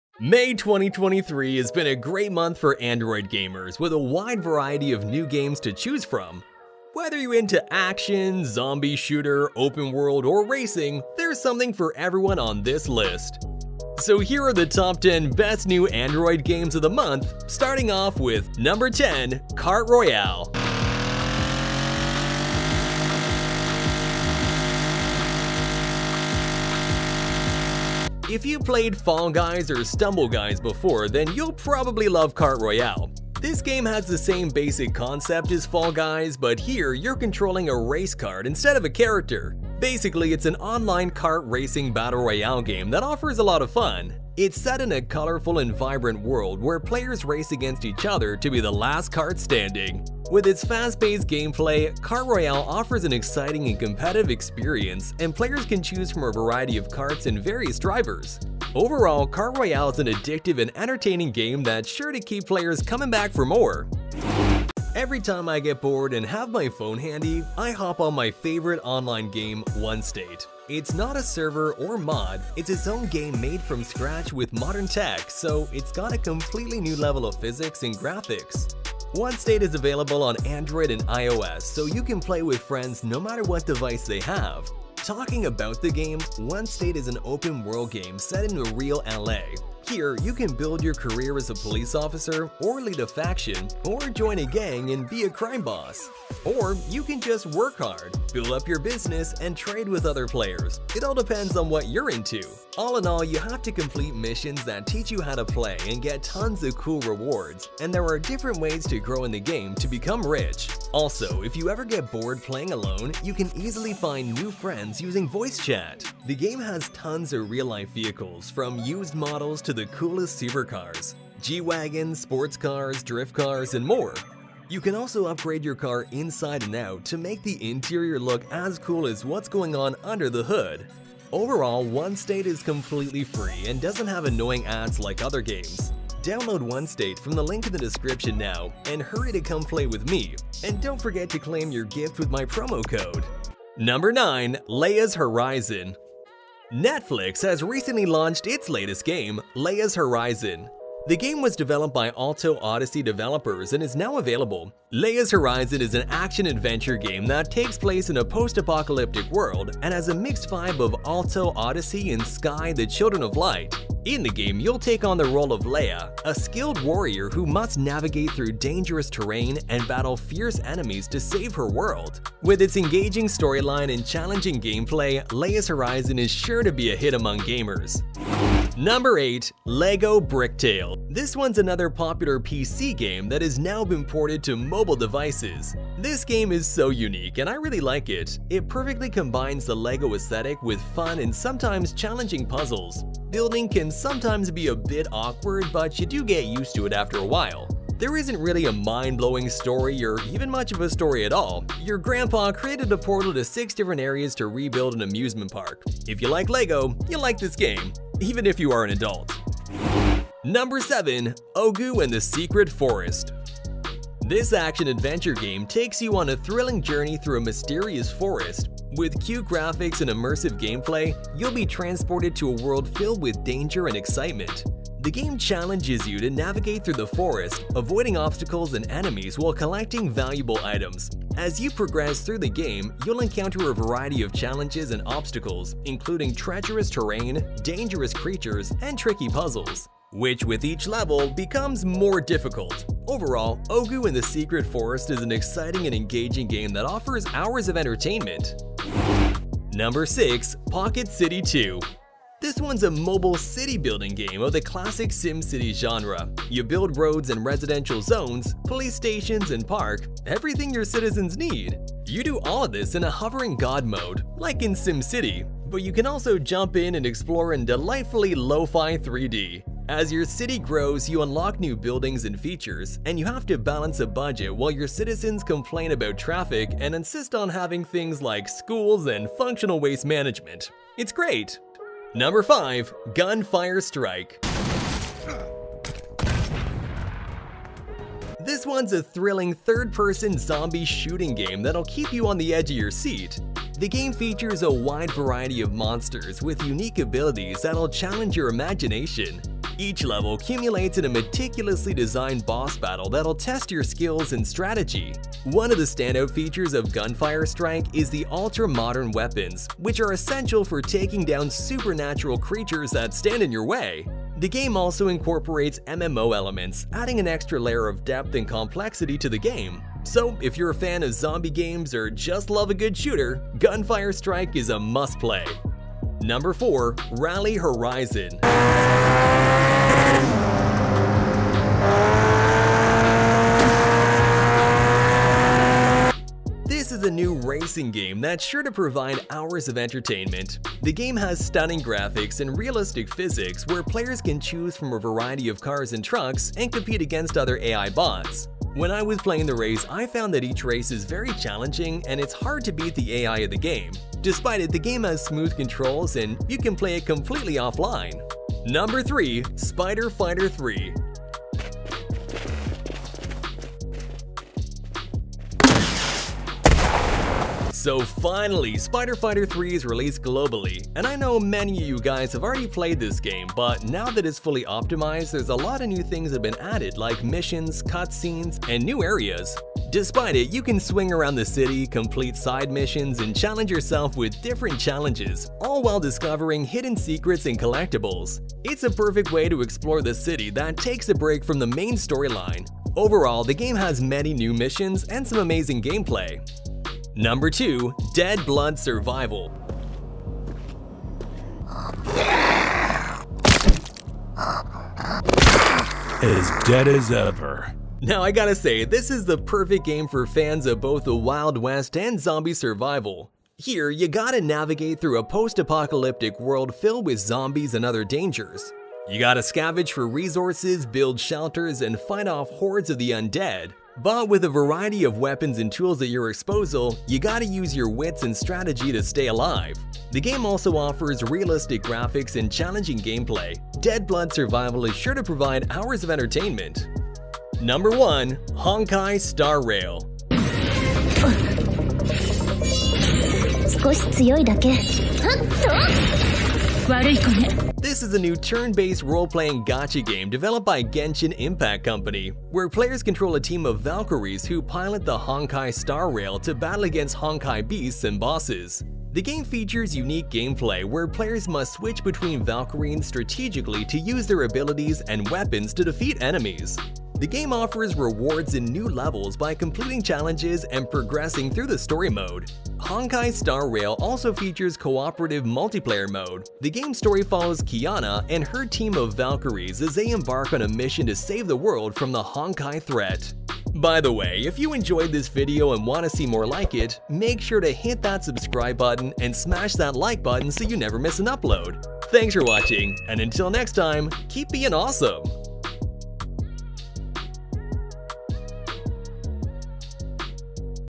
Top 10 New Games for Android & iOS of May 2023 | Best Mobile Games!
Hey guys in today's video we gonna take a look on top 10 best new Android & iOS games of the month, In this video we have covered 10 best mobile games which was released recently.

Game list